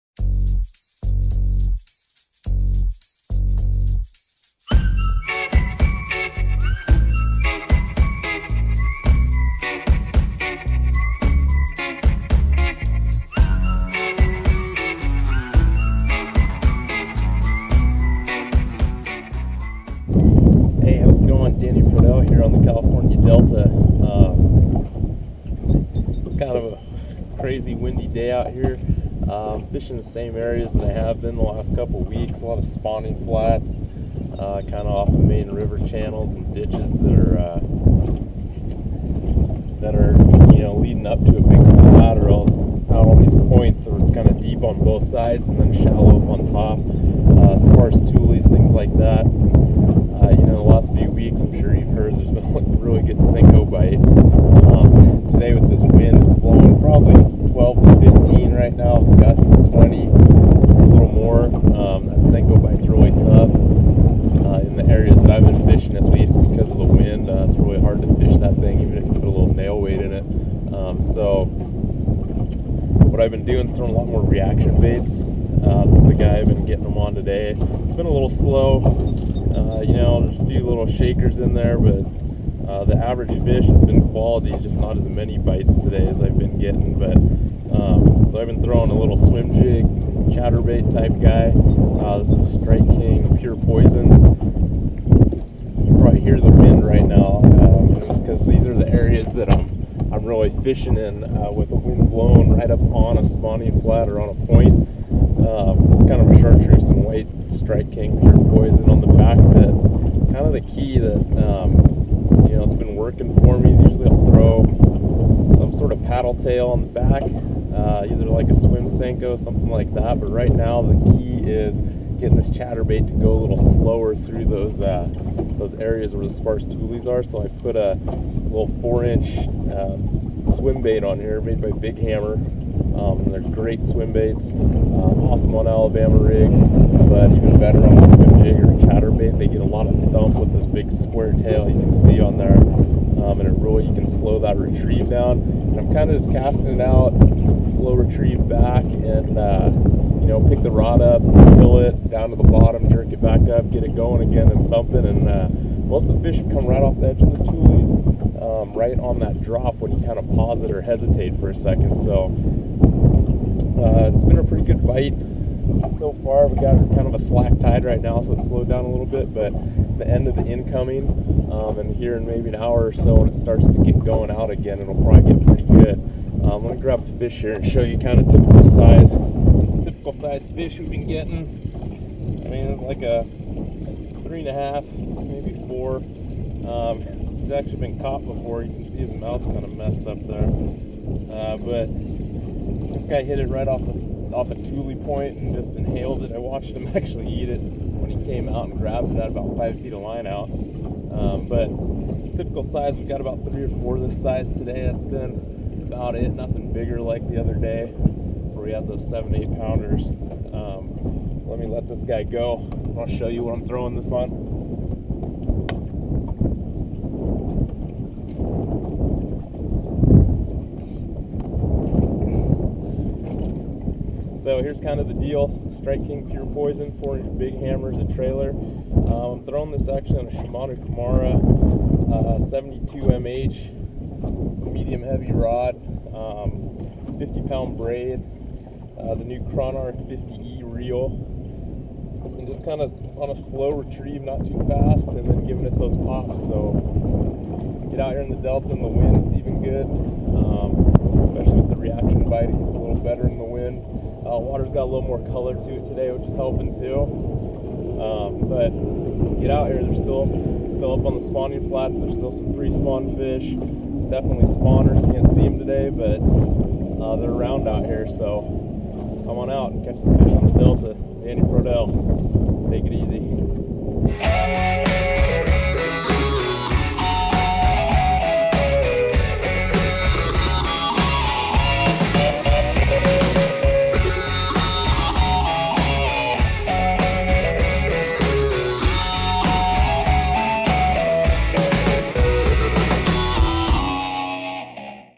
California Delta Report April 23, 2013
Get out on the water and get some big bass on the Dirty D! The bite is good and will only be getting better with this warming weather. Hope the tips and patterns I talk about in this video help you get on the fish up here in nor cal. Aloha